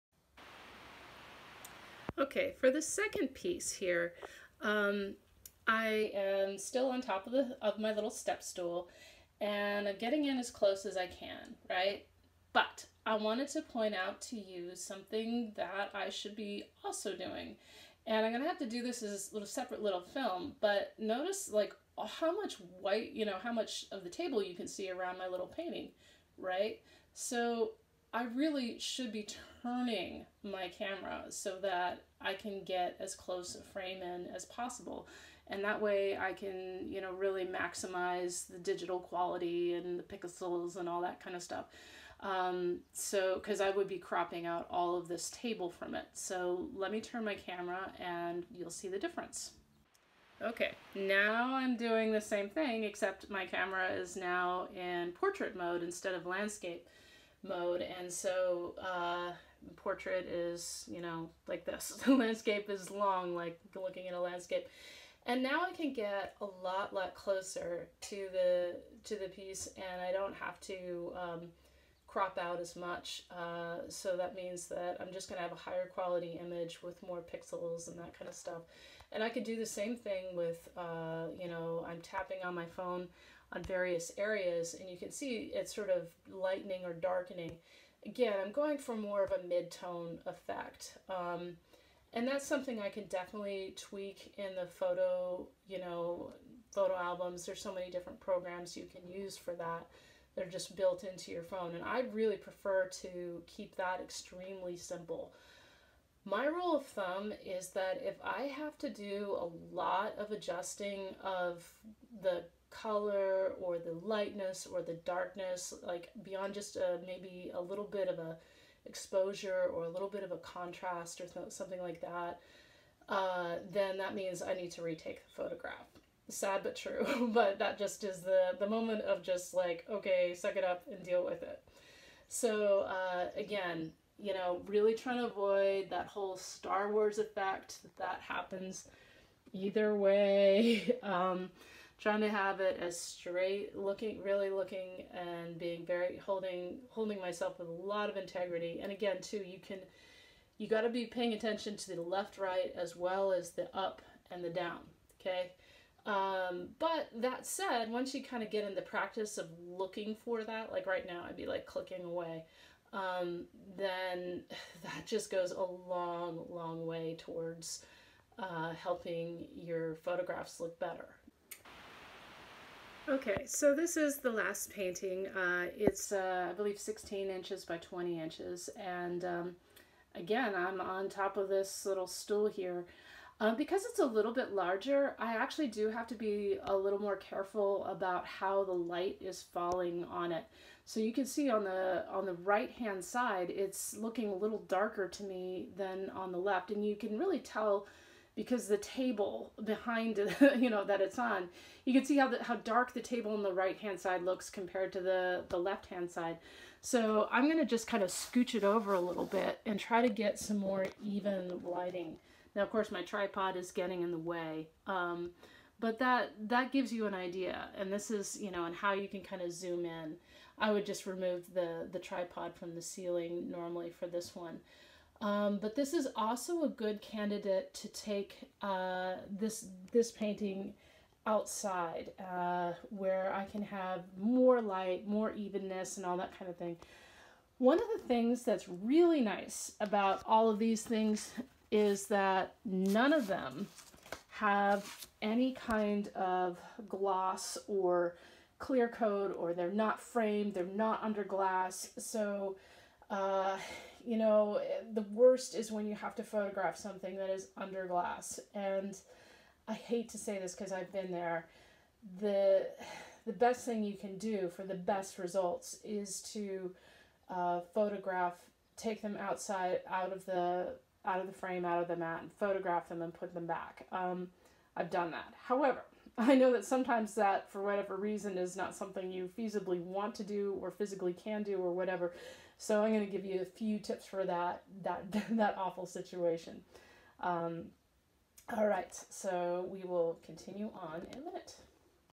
02 How to Photograph Your Work Melissa Klein portrait vs landscape framing no gloss
Framing of art within the camera and being aware of portrait format vs. landscape to get the best quality picture.